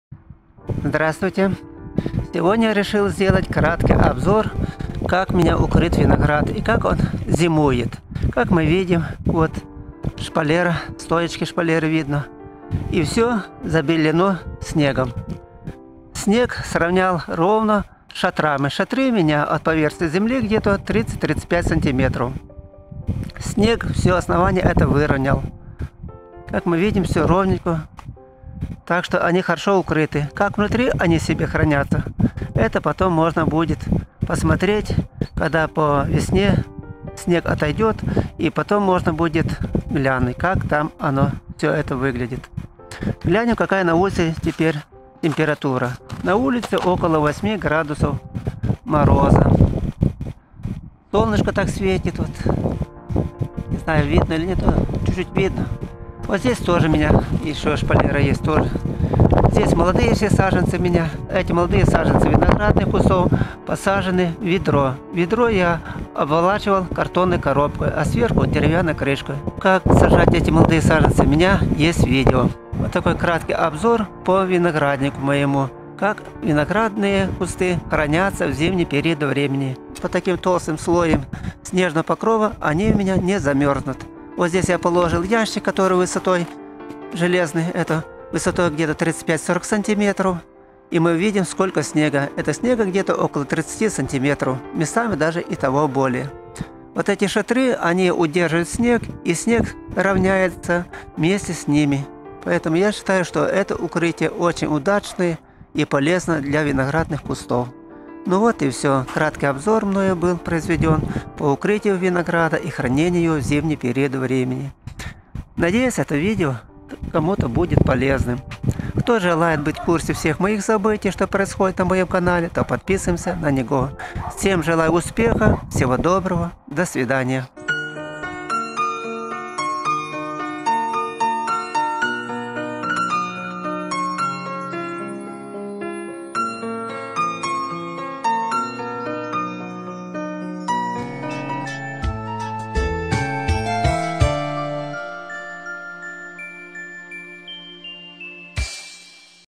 Виноградник зимой. Обзор виноградника
Виноградник зимой. С приходом зимы необходимо произвести обзор виноградника и при необходимости укрыть виноград снегом,что бы в мороз не вымерзли кусты.Подготовка винограда к зиме –это наиболее важная работа на винограднике так как от того насколько правильно мы обеспечим ему зимовку, будет в дальнейшем зависеть скорость его развития, количество и сахаристость ягод винограда. Если уход за виноградом был постоянен, то по наблюдениям нашим мы своевременно произведём  те или иные работы на винограднике, для благополучной зимовке его.Обзор виноградника зимой,что бы укрытие винограда  было защищено от морозов и других не благоприятных условий.Наблюдательный процесс включает в себя осмотр виноградника по укрытию его. Таковой работы на винограднике не предвидится как на протяжении всего теплого периода времени.Правильная подготовка винограда к зиме позволит защитить это растение от вымерзания и подмачевания лоз винограда от талых вод весной. Поэтому, каждый виноградарь старается на своем винограднике что -то использовать новое к защите лоз винограда от всяких не предвиденных обстоятельств. Работа на винограднике зимой-это осмотр его и защита от грызунов и погодных условий. Обзор на винограднике зимой делаем внимательно ,что бы  укрытие винограда зимой было надлежащим образом,и при необходимости устраняем обнаруженные дефекты своими руками.